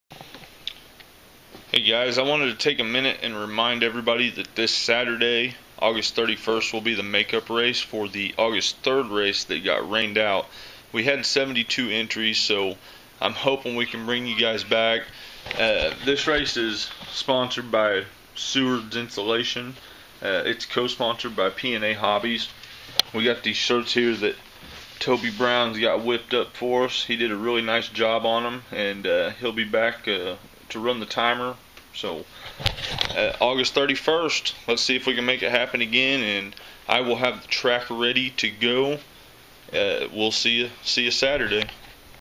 Aug. 31 Race @ SVRC "Summer Shootout 2013"
Hey guys, just trying to get you all hopped up for the summer shootout going down at svrc on aug. 31. If you have any questions search me on fb for Spring Valley Short Course and Ill get it hooked up. SVRC Out!